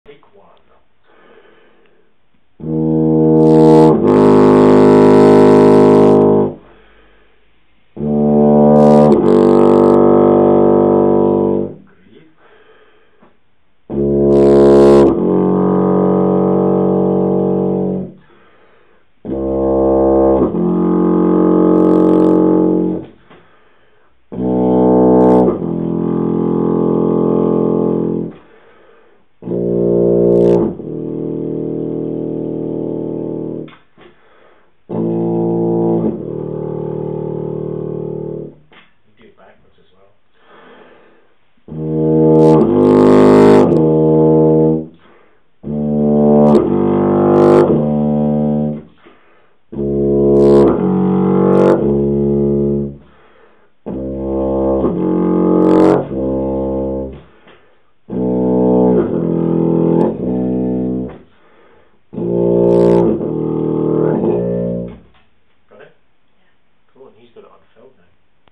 James Gourlay - low register exercise for Tuba
James demonstrating an exercise of air and embouchure strengthening for my wee bro Andy.

Descending 5ths without the aid of the 4th valve until the extreme low register.